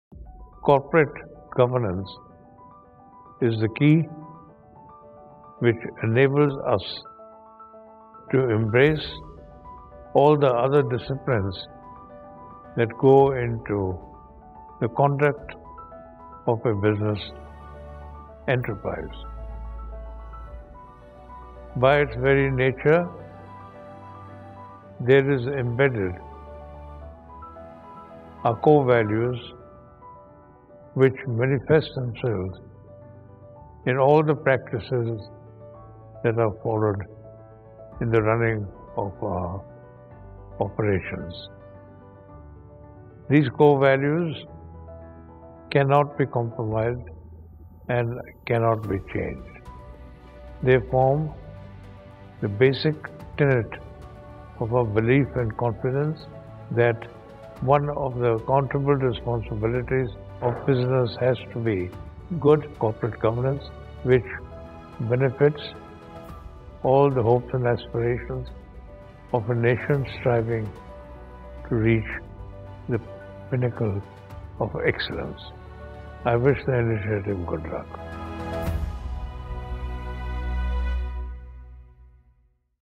Keshub Mahindra On the Importance of Corporate Governance | Annual Review FY17 | Mahindra Rise
Keshub Mahindra, Chairman Emeritus, Mahindra & Mahindra Ltd., talks about the importance of corporate governance.

To find out more, take a look at our Annual Review 2017